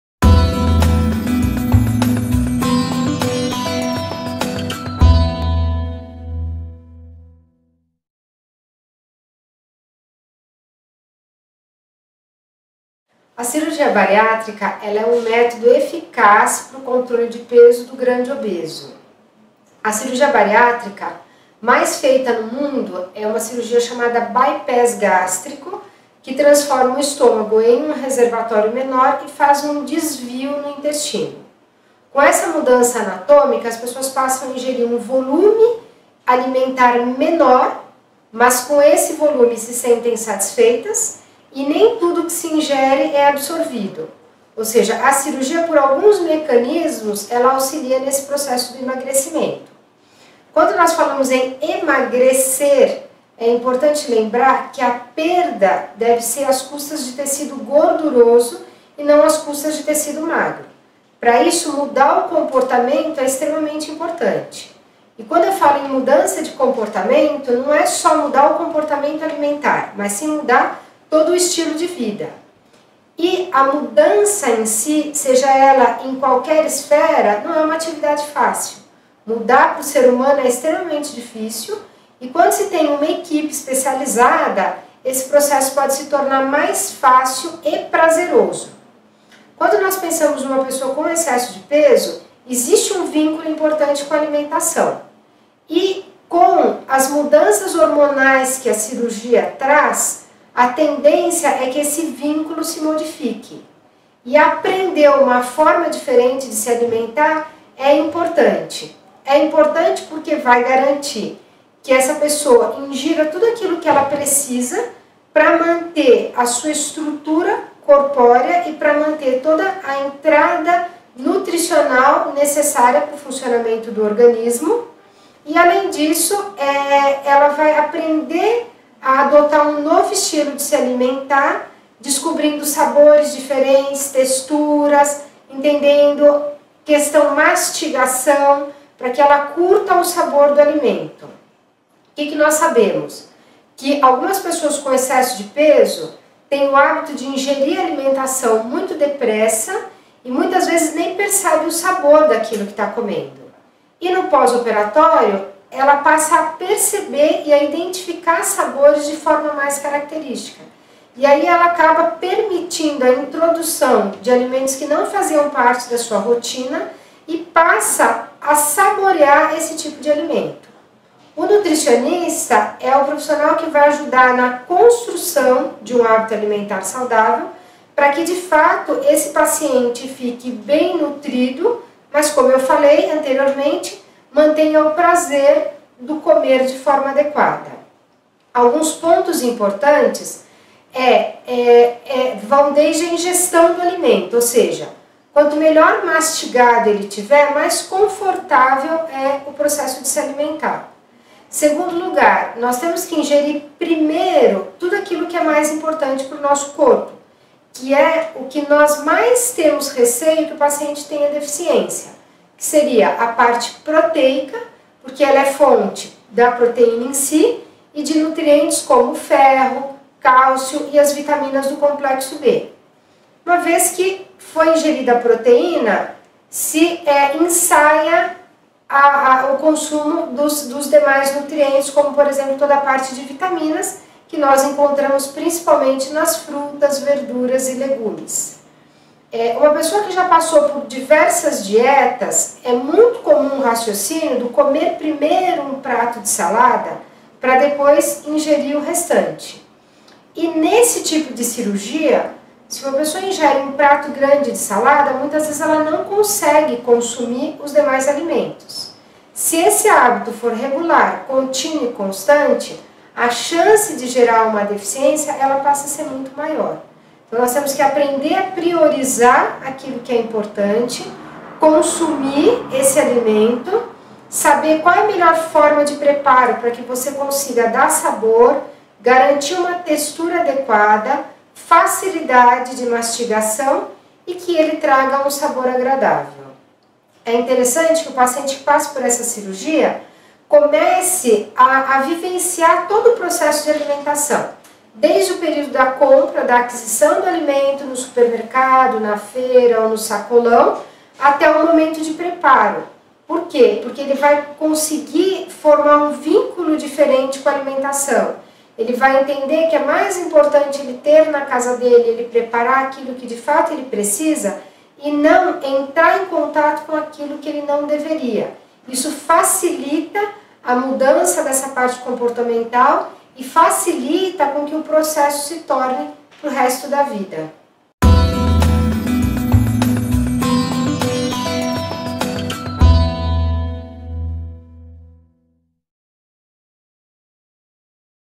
Hábitos alimentares após Cirurgia Bariátrica (Vídeo Dicas)
Dra. Alessandra Coelho explica a questão dos hábitos alimentares após cirurgia bariátrica.